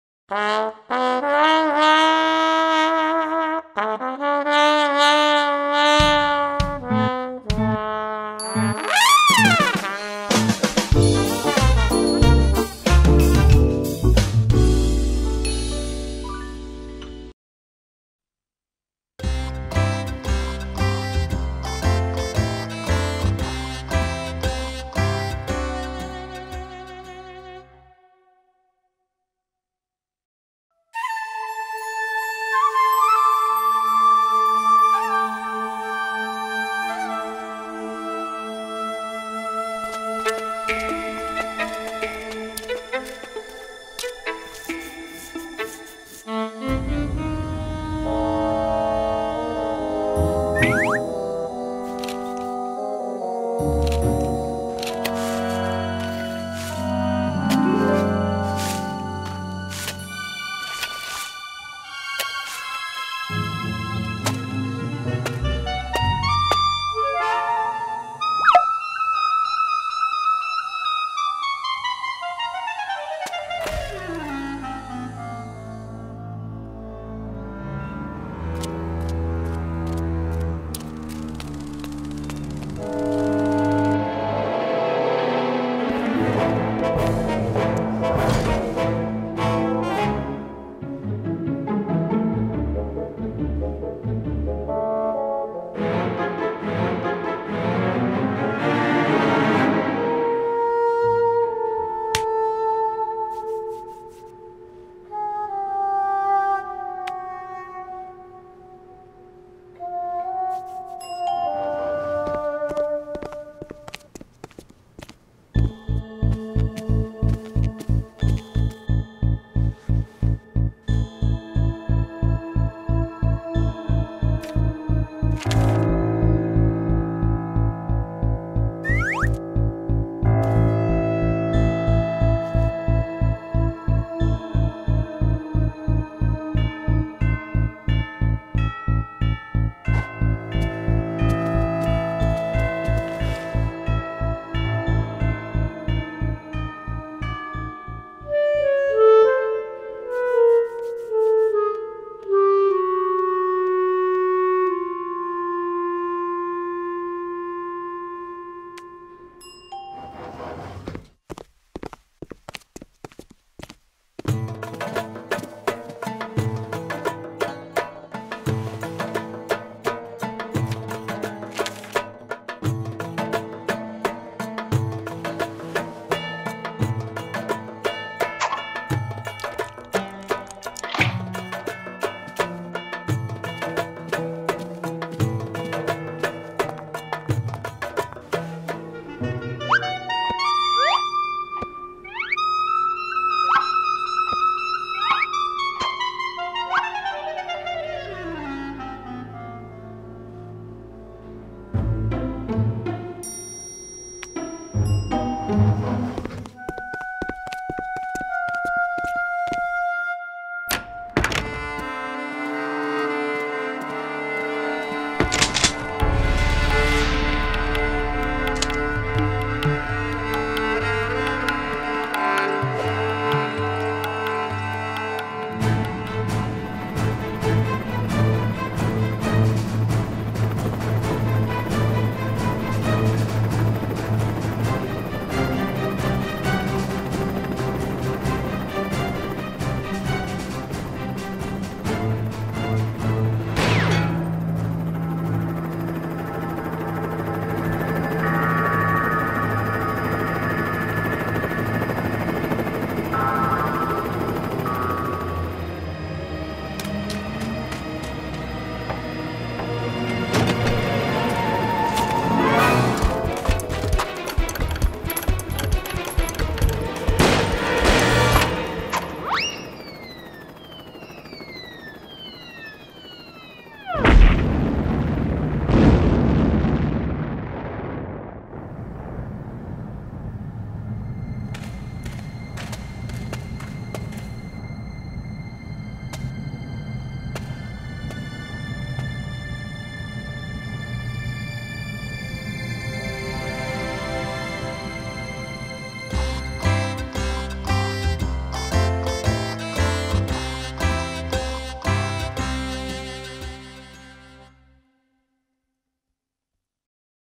Gas & Pash, Episode 2, Nuclear Bomb
In this episode, Gas and Pash discover a bomb in the desert! They try to use this weapon to again reach wealth!